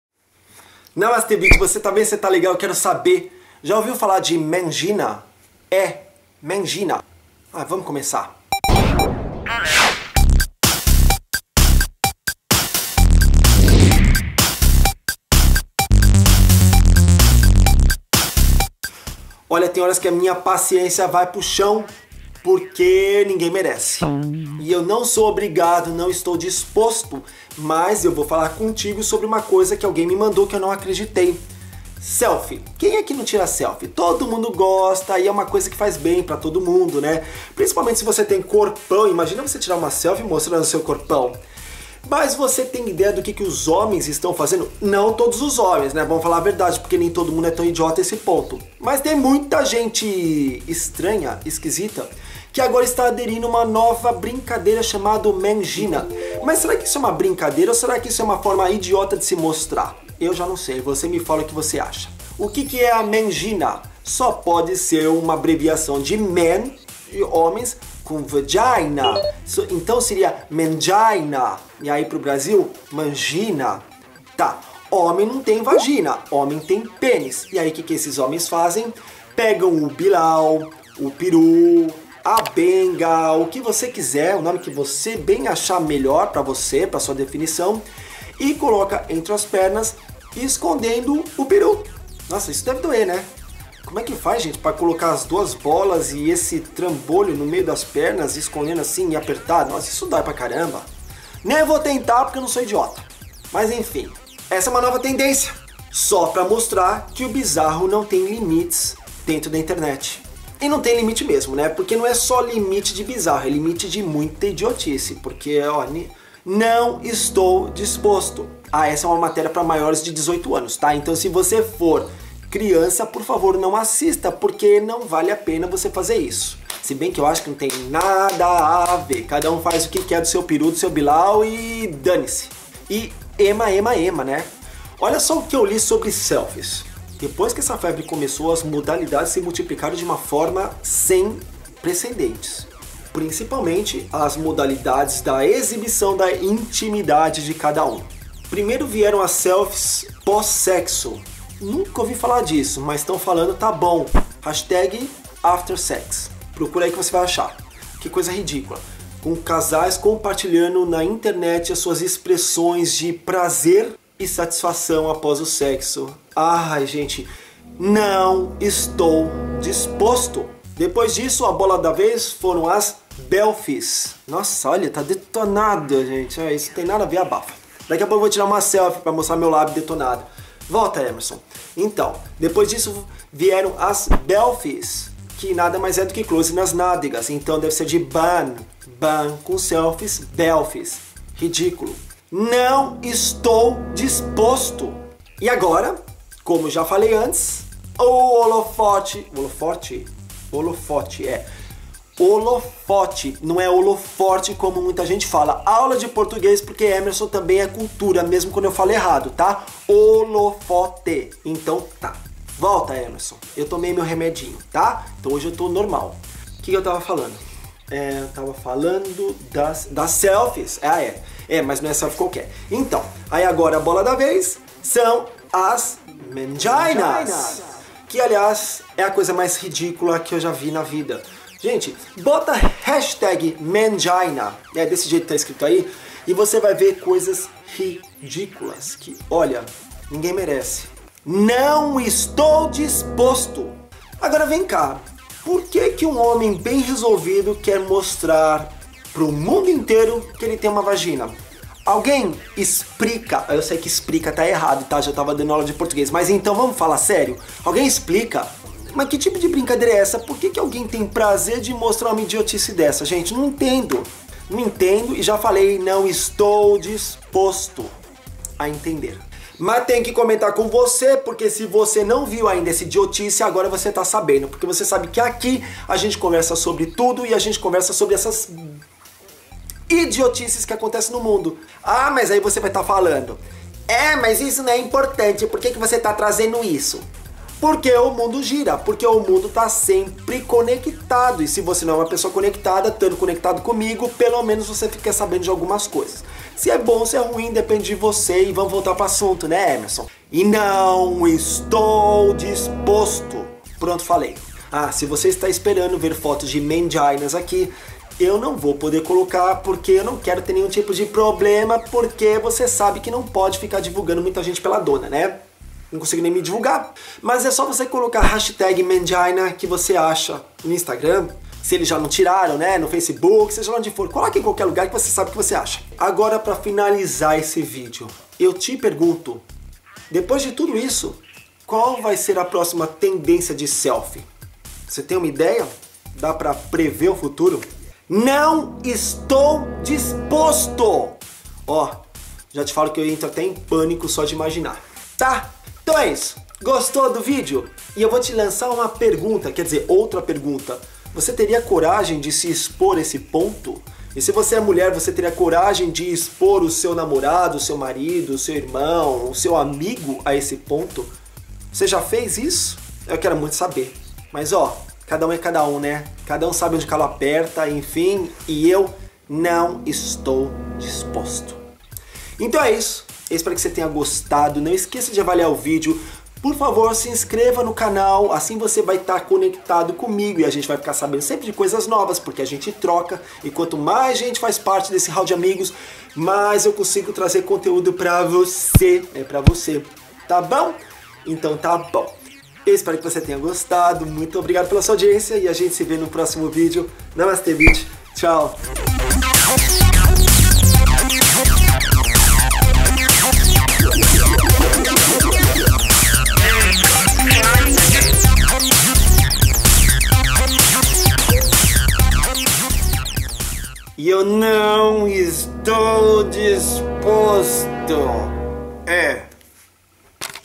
Homens de Vagina - A nova moda chamada mengina - EMVB - Emerson Martins Video Blog 2015
Já imaginou se essa moda pega? O que você acha de homens com H maiúsculo de vagina? Saiba tudo sobre mangina!

DIvirta-se!

Namastê Bitch!